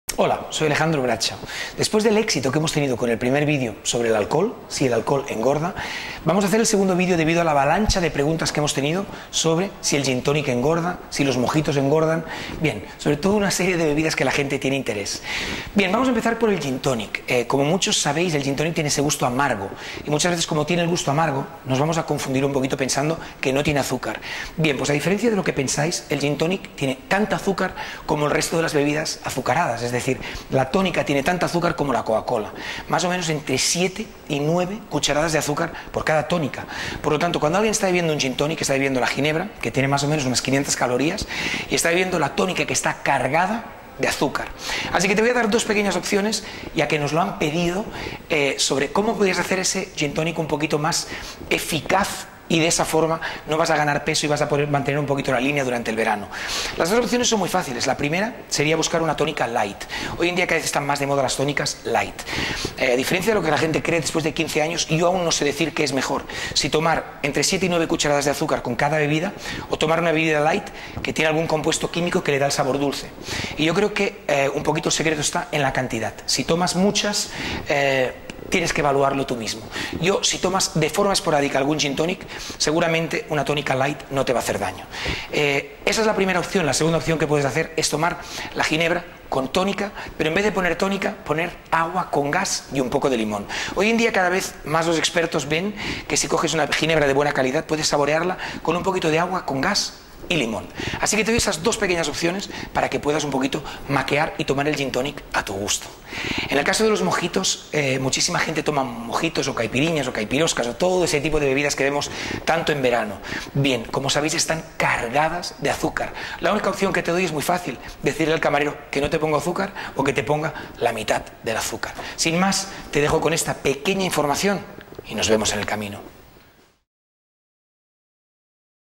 Cuida tu cuerpo | ¿El Gin Tonic engorda?
El Gin Tonic tiene tanta azúcar cómo las bebidas azucaradas.